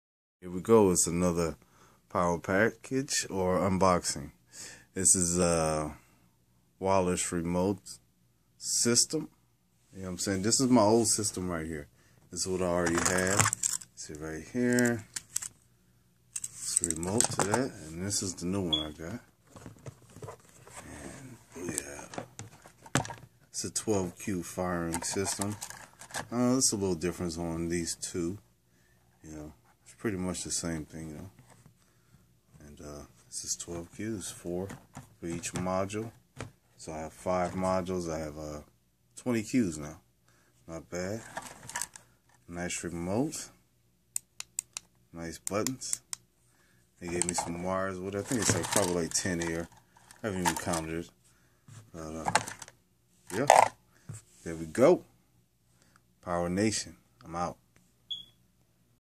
12 cue firing system
Showing 12 cue firing system. And last years 2 -4 cue system. They are reliable  and is programmable for more modules and most of all cheap.